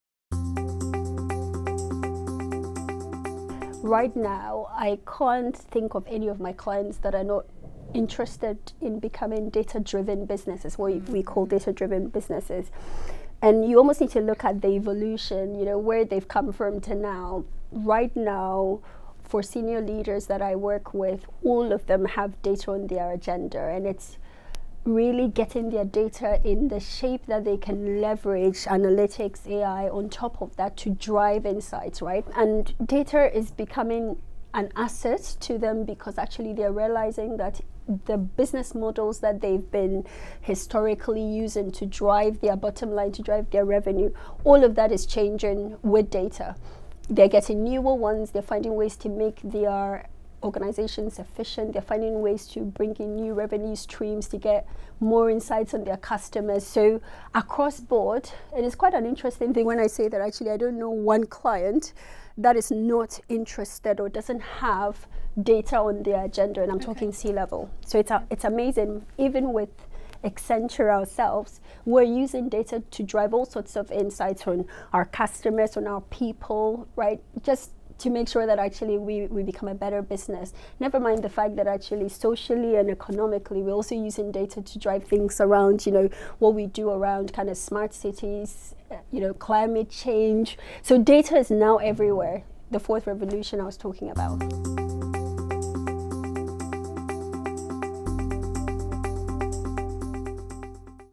How to become a data-driven business
Nearly every company Belinda Djamson works with wants to become "data-driven". What's her advice to turn their data into business assets? Accenture's Data and Analytics Management Lead, explains. Filmed at The Studio 2019.